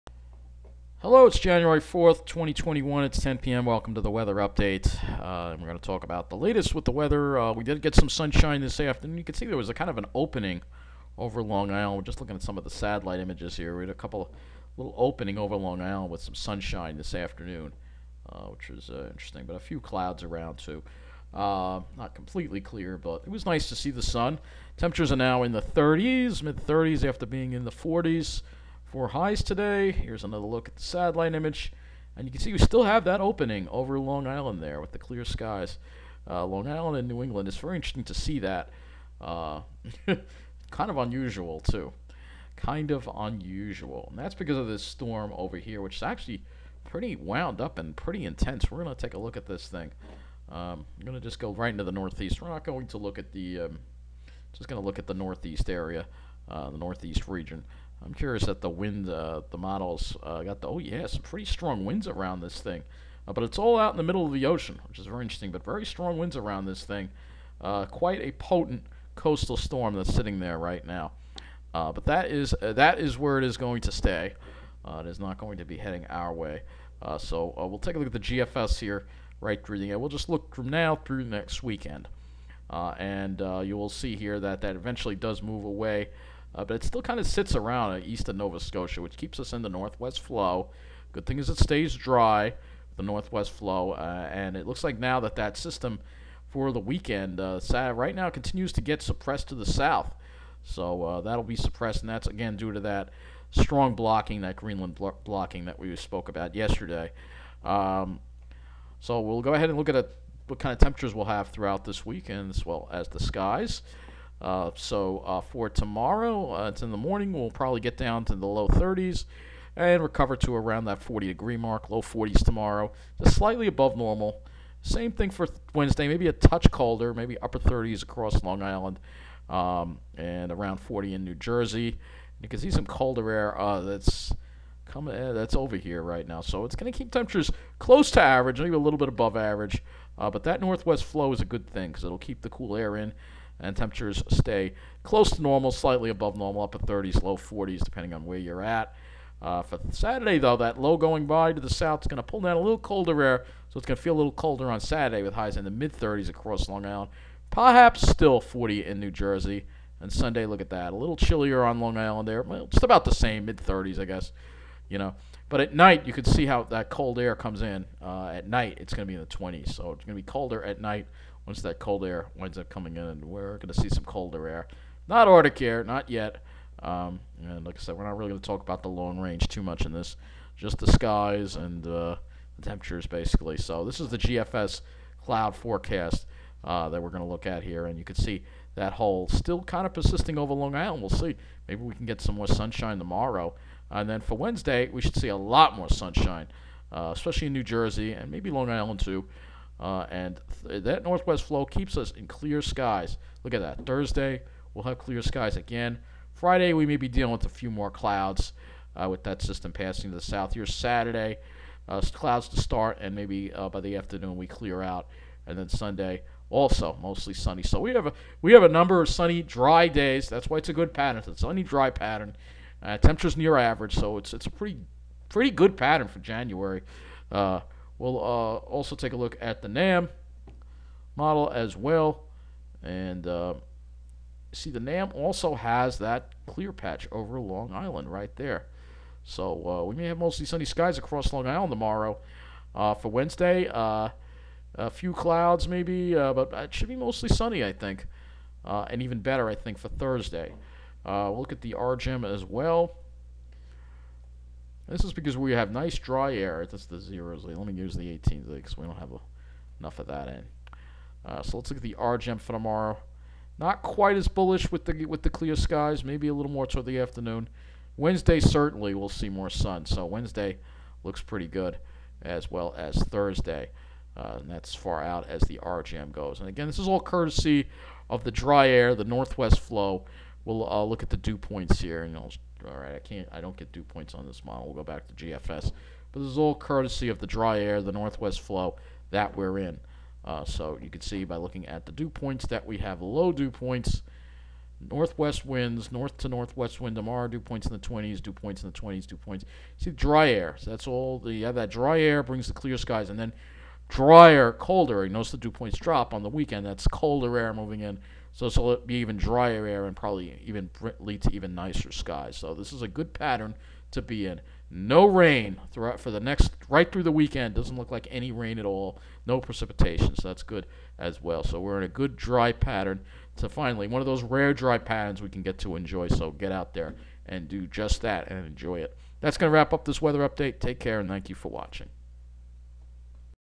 Weather Update: Great Stretch of Weather Ahead!
Sunshine, temps around average, and no precipitation likely through this week and into the weekend!